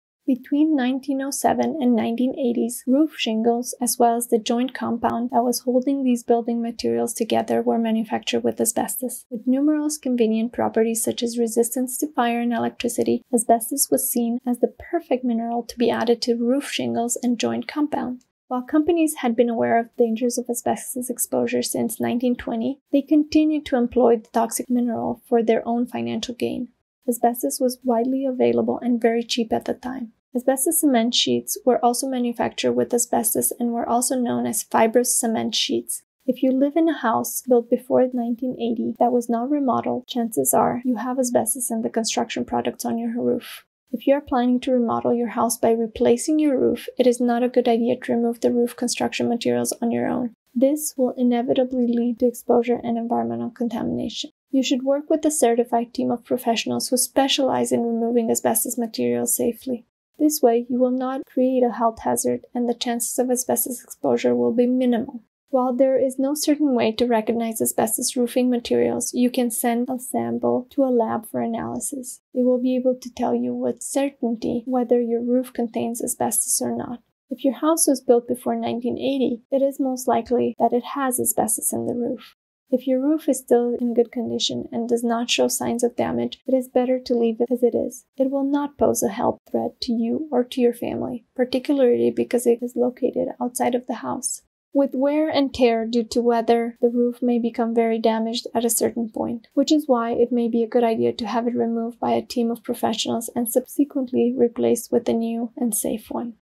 How to Identify Asbestos Roofing Material
If your house was built before 1980, it most likely has asbestos in the roof. However, if your roof is still in good condition and does not show signs of damage, it is better to leave it as it is, as it will not pose a health threat to you or to your family, particularly because it is located outside.

Are you worrying asbestos might lurk in your house and would like to know with certainty how safe your living space is?